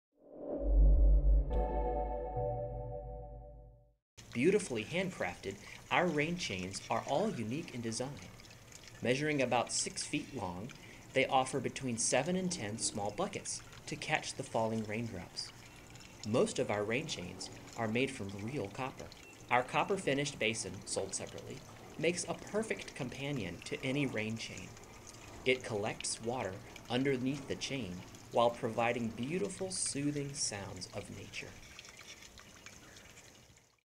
Rain Chains & Basin
Beautifully handcrafted, our rain chains are all unique in design. Measuring about 6 feet long and offer between 7 and 10 small buckets to catch the falling raindrops. Most of our rain chains are made from real copper.
Our copper finished basin makes a perfect companion to any rain chain. It collects water underneath the chain, while providing beautiful soothing sounds of nature.